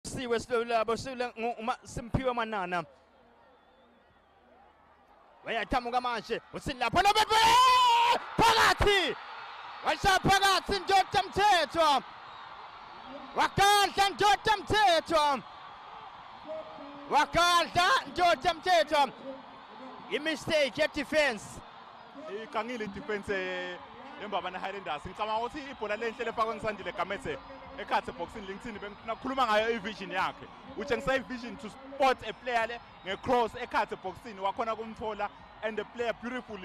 A Goal for Young Buffaloes || The Ingwenyama Cup Final 2019
Young Buffaloes will face Mbabane Highlanders on the Ingwenyama Cup final to be held on the 17 March 2019, at the Somhlolo National stadium. An event that will be graced by Their Majesties.

Young Buffaloes beat Ingwenyama Cup treble Champions Mbabane Swallows 2-0 to reach their second Ingwenyama Cup final.

Mbabane Swallows have won all the editions of the tournament since its inception I n 2016.

Mbabane Highlanders beat Malanti Chiefs 2-0 to reach their first final in 7 years, The last time Mbabane Highlanders were in a cup final was in 2012 when they lost to the Lloyd Mutasa coached Green Mamba 3-1 in the Eswatini Bank Cup with 2010 being their last cup final triumph when they hit a double.